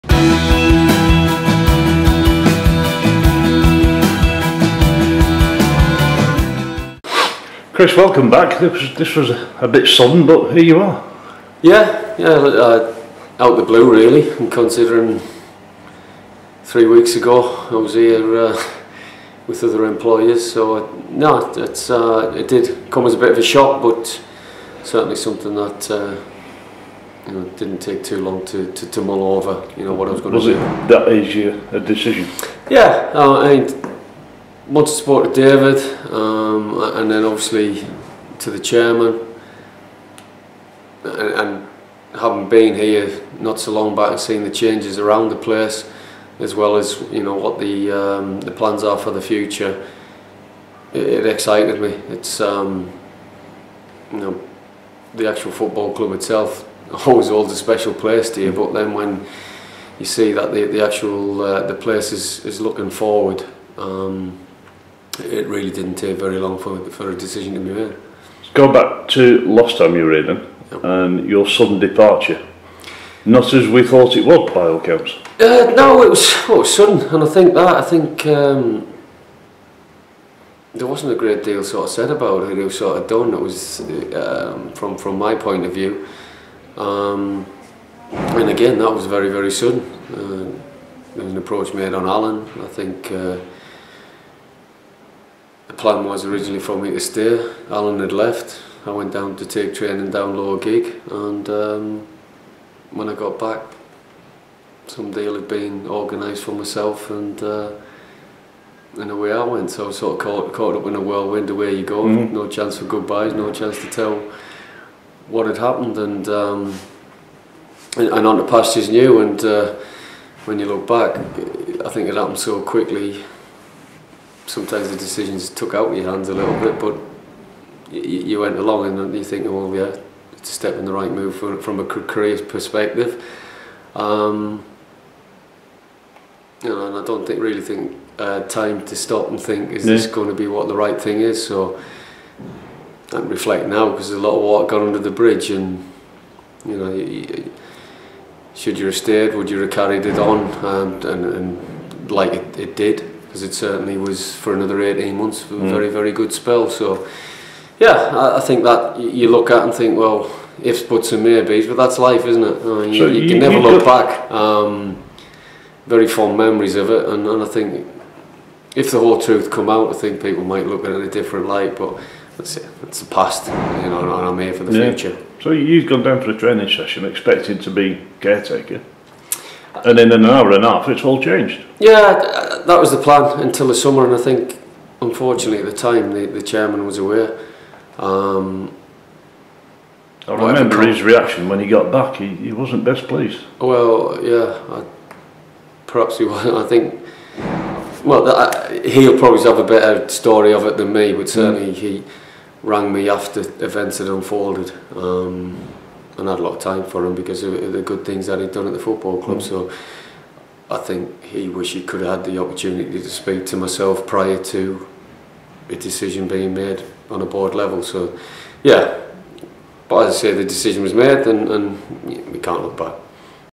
Bury FC: Chris Brass
Chris Brass on arriving at the JD Stadium